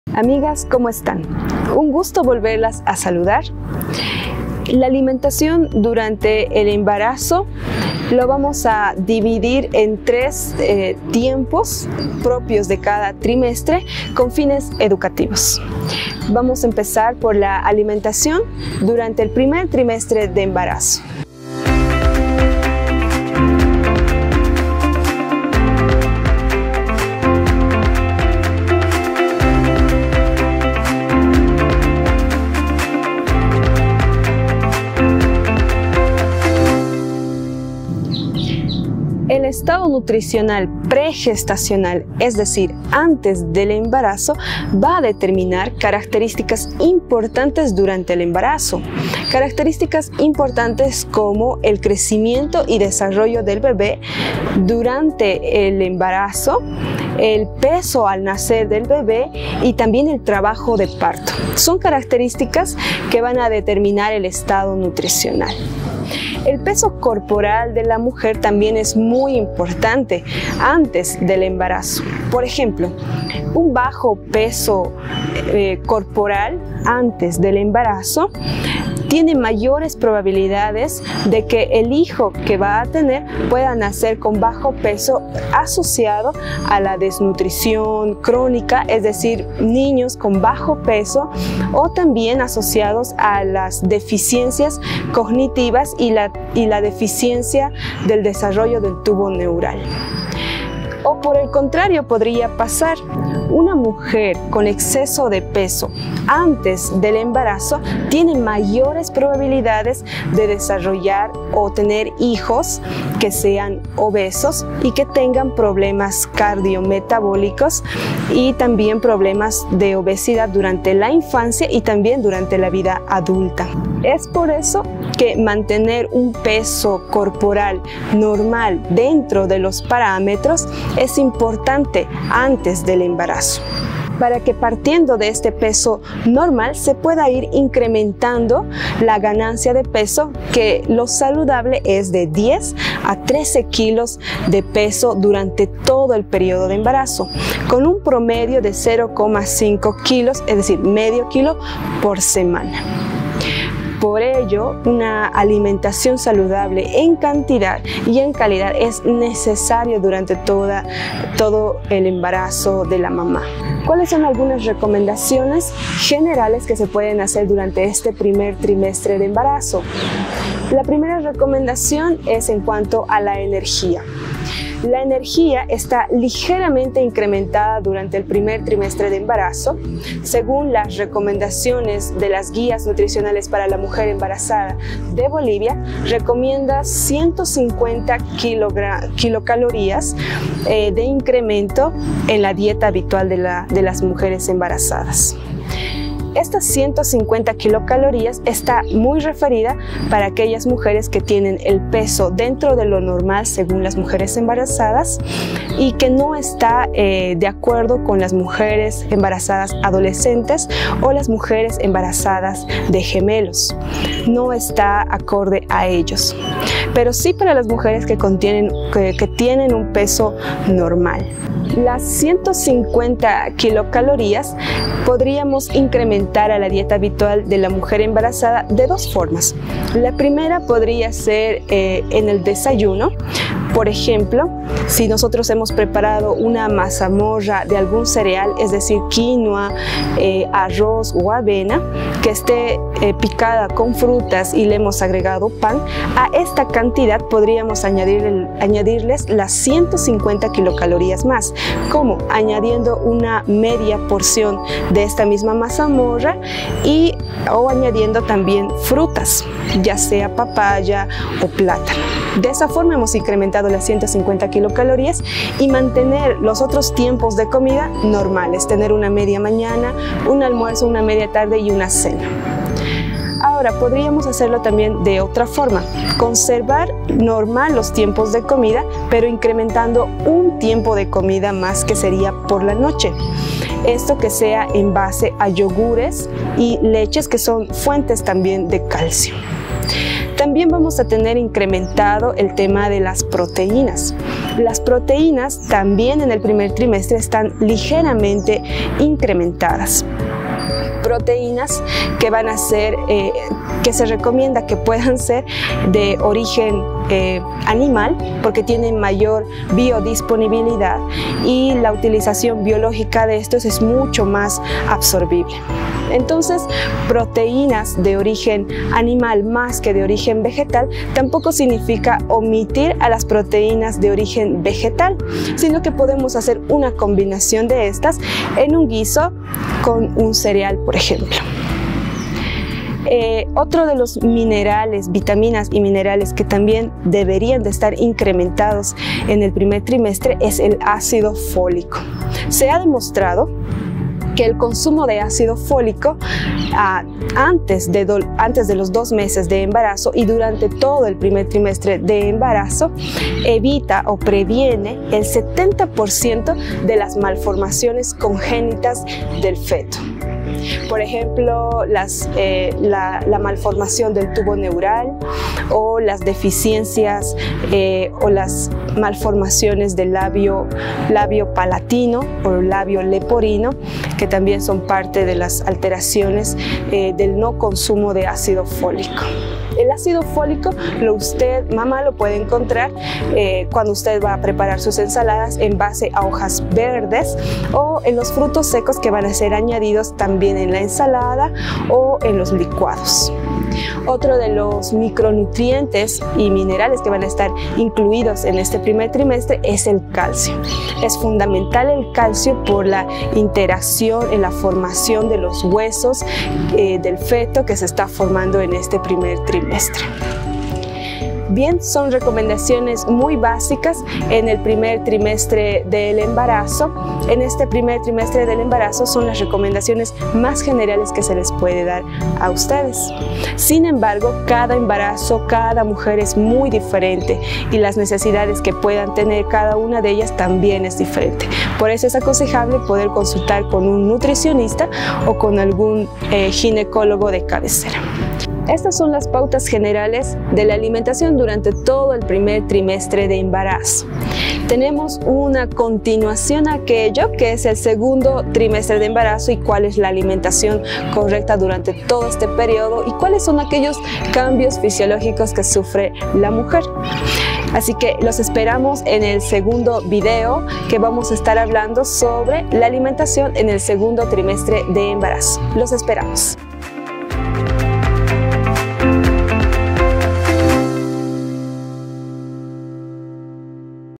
¿Cómo se debería alimentar una Madre en el primer trimestre de embarazo? (Parte 1)
El embarazo es una bendición para todas las mujeres, pero algunas veces, especialmente cuando seremos madres primerizas, no sabemos cuáles son los cuidados que tenemos que tener, en especial en la alimentación ¿Cómo se debería alimentar una Madre en el primer trimestre de embarazo?, Acompáñanos para que junto con la Nutrióloga Maribel Salvador podamos dar respuesta a estas y otras preguntas.